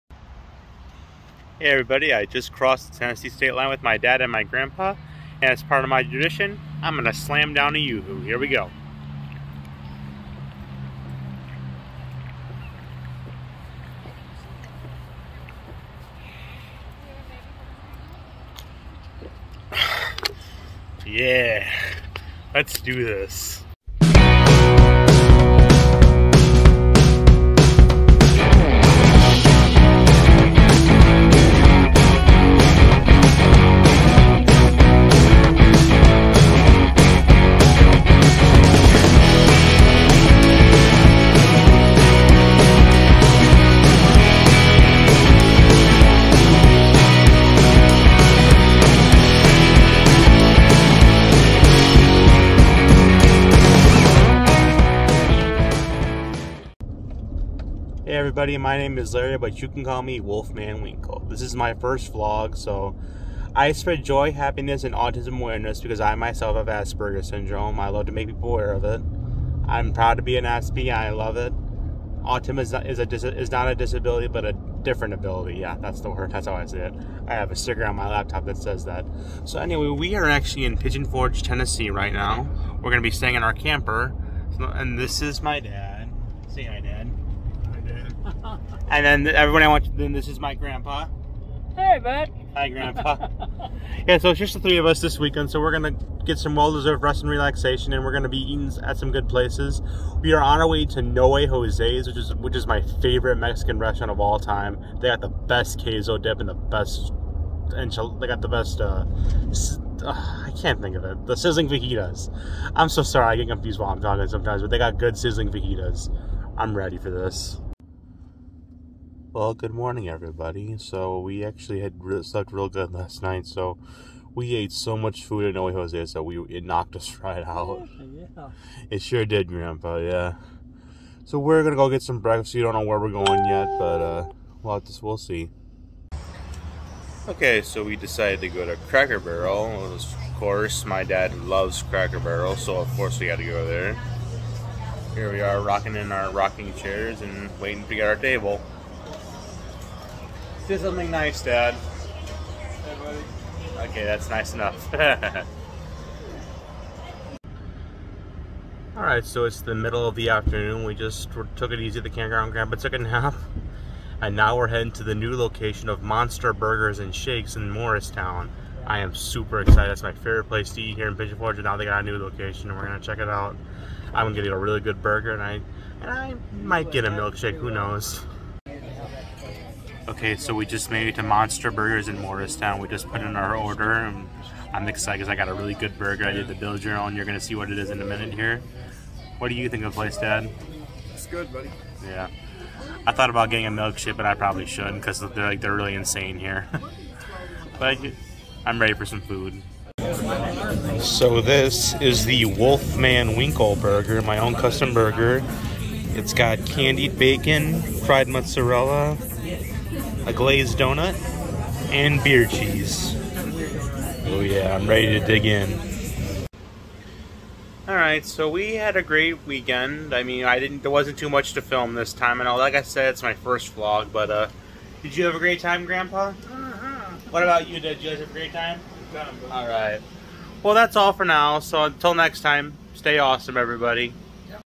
Camping In The Smoky Mountains/The Wolfman Winkle Burger
My very 1st vlog! I know it's very short, but I was new to the whole vlogging thing. This was back when my Dad purchased our new camper, and my Grandpa came out with us to stay! We went to Monster Burgers & Shakes in Morristown, which is unfortunately closed for good. My whole vlogging adventure started with me slamming down a Yoo Hoo at the Tennessee State Line.